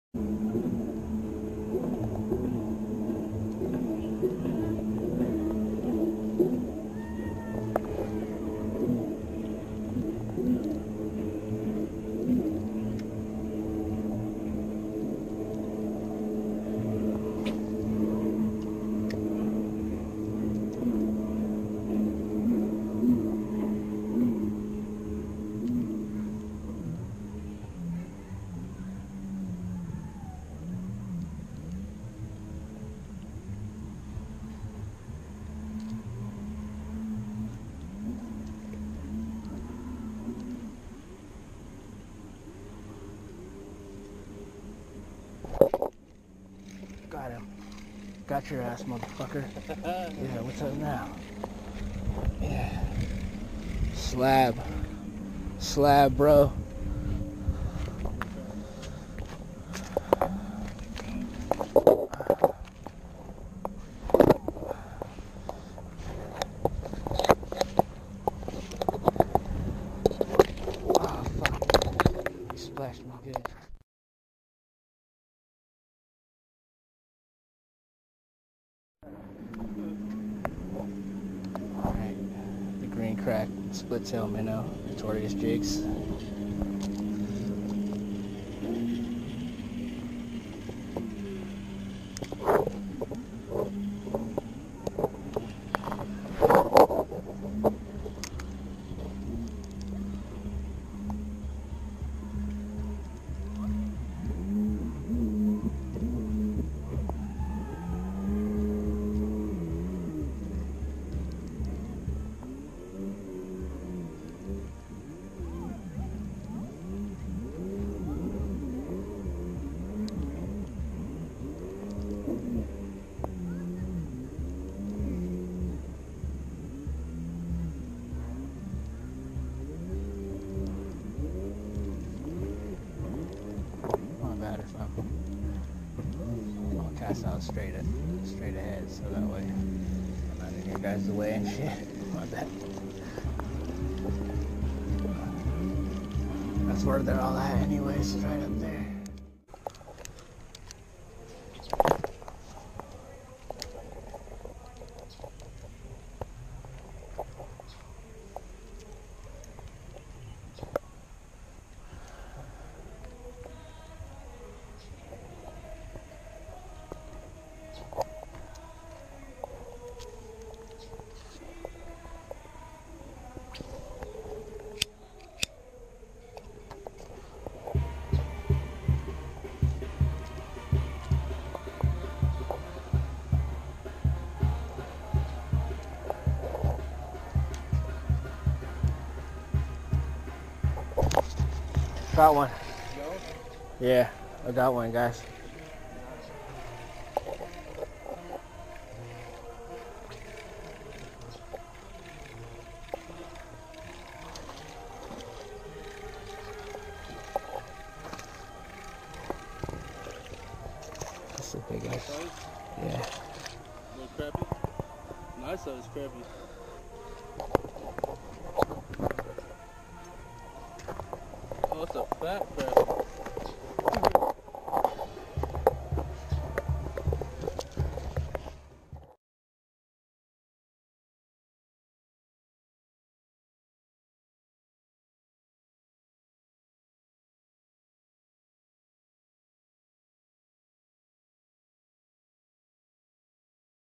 Sunday Slabs w/Notorious Custom Jigs and Kaotic Jighead AUG 19, 2018
Slabbin' on a Sunday with some friends that I happened to run into at the lake, Notorious pearl/black flake and green crack split tail minnow were the ticket.. 🎣🎥📺